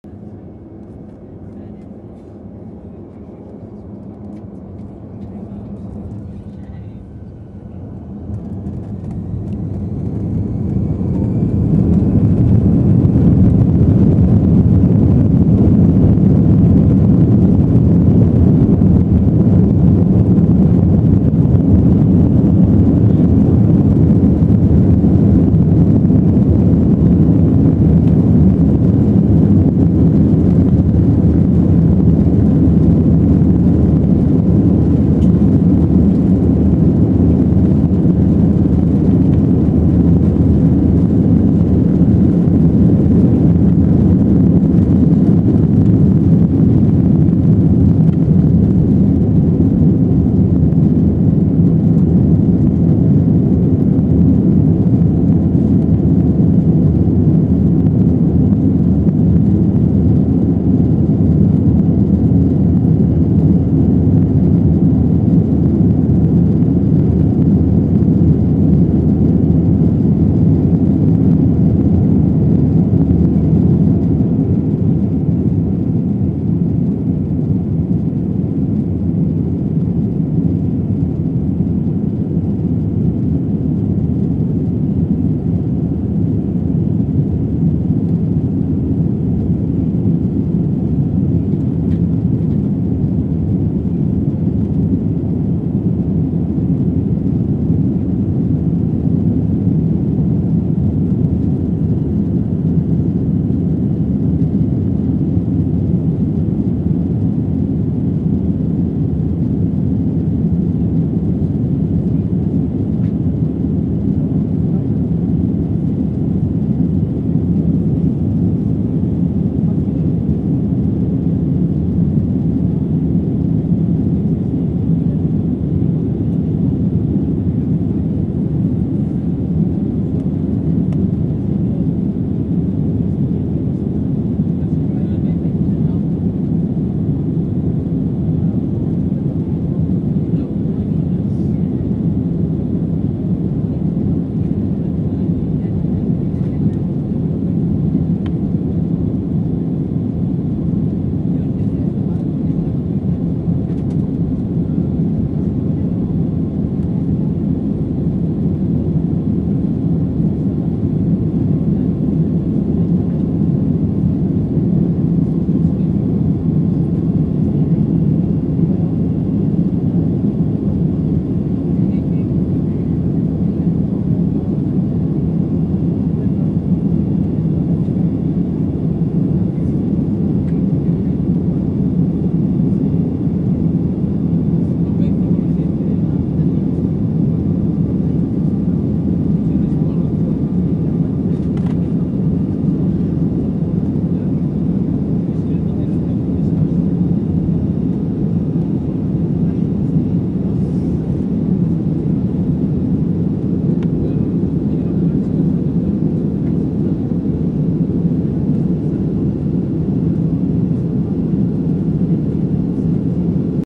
Solomon Airlines A320 Takeoff in Honiara, Solomon Islands
Honiara - Brisbane, A320 (H4-BUS) Solomon Airlines, (13/01/13)